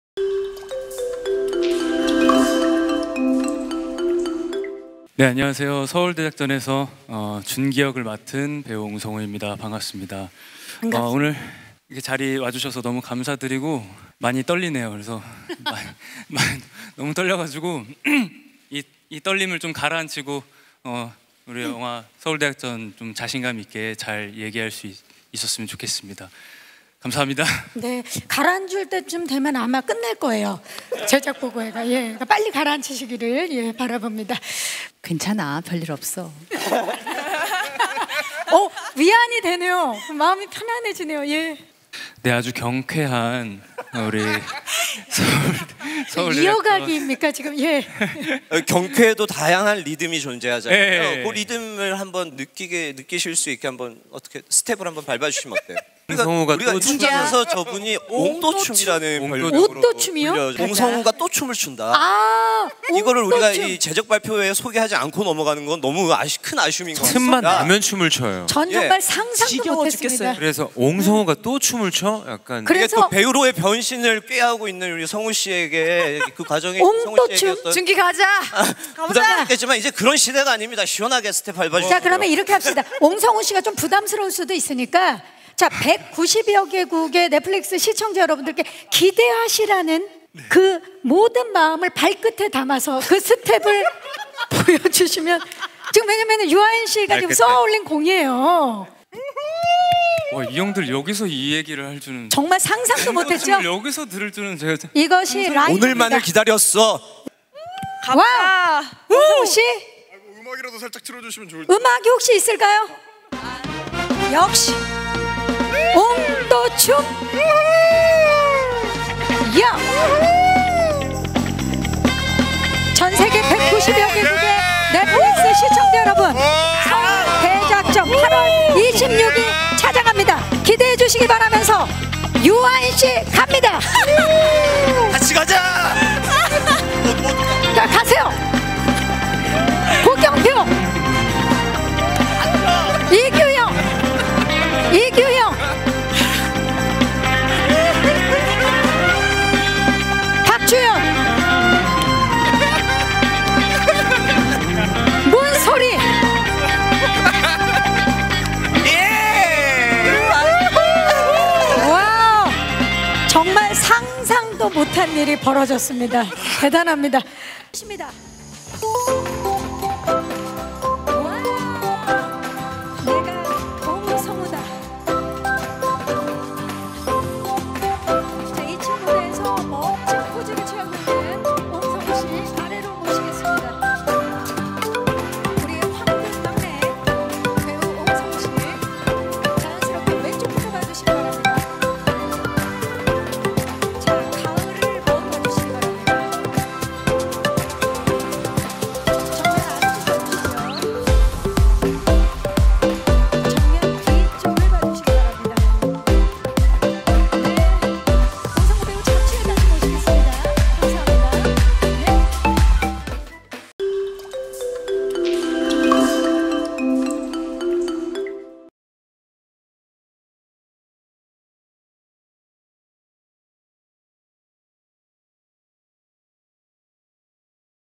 HK영상｜'서울대작전' 옹성우, 영화를 닮은 경쾌한 춤 선보여
배우 옹성우가 23일 오전 서울 강남구 그랜드 인터컨티넨탈 파르나스에서 넷플릭스 영화 '서울대작전'(감독 문현성, 제작 앤드마크스튜디오) 제작보고회에 참석해 춤을 선보이고 있다.

'서울대작전'은 1988년 아메리칸 드림을 꿈꾸는 상계동 슈프림팀이 거부할 수 없는 제안을 받고 VIP 비자금 수사 작전에 투입되면서 벌어지는 카체이싱 액션 질주극으로 오는 26일 넷플릭스에서 공개된다.